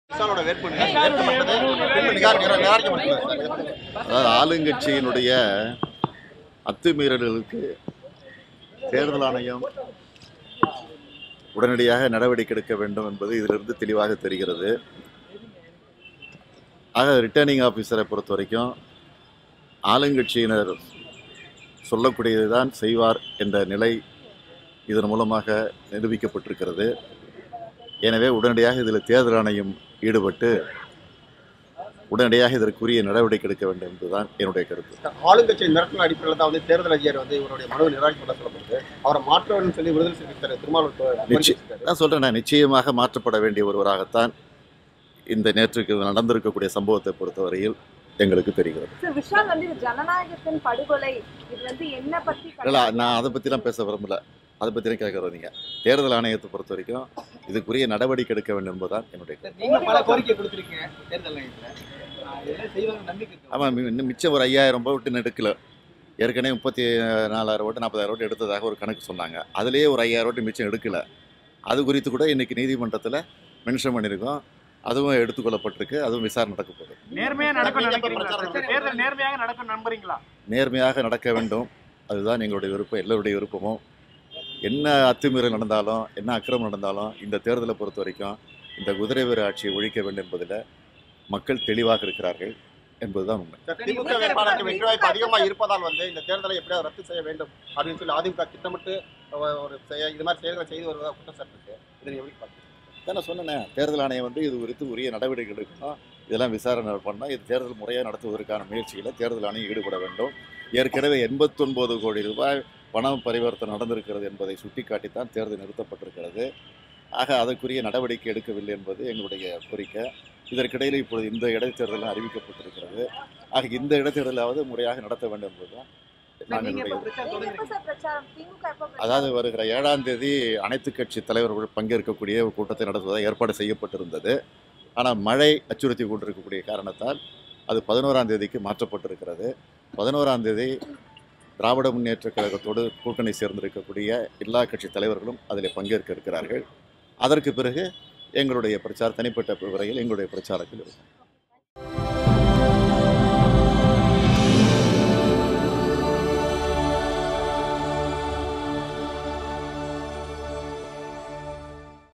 Replace returning officer in R.K. Nagar : Thalapathi MK Stalin
DMK working president and leader of opposition M.K.Stalin said that the the controversy over the rejection of actor Vishal’s nomination papers showed the Returning Officer would do only what the ruling party told him to do. He also urged the Election Commission to investigate into the matter. Connect with Thalapathy Stalin on:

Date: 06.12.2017